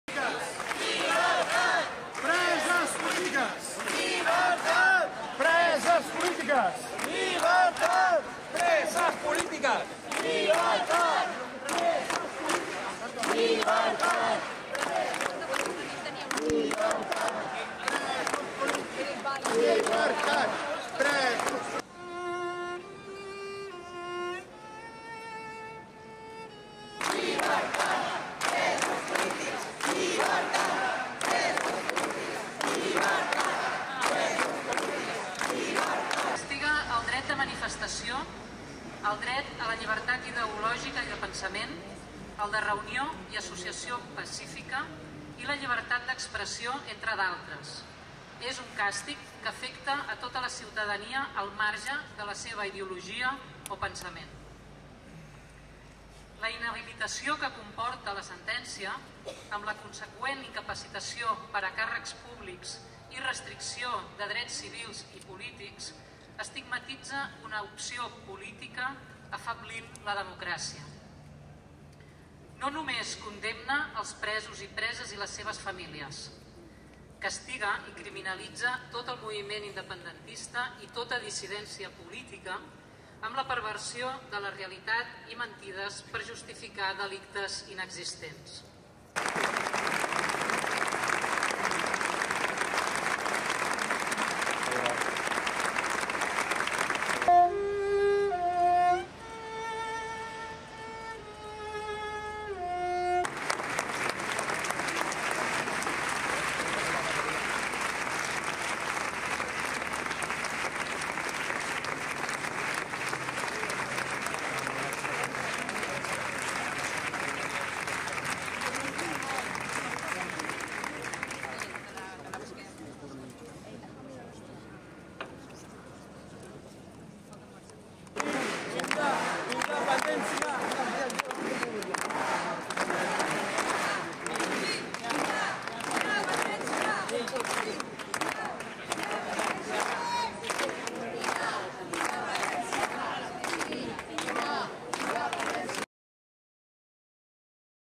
Kataluniaren auziko epaia: Senideen adierazpena
| Marina Garces filosofoak presoen senide guztiek adostutako adierazpena irakurri du.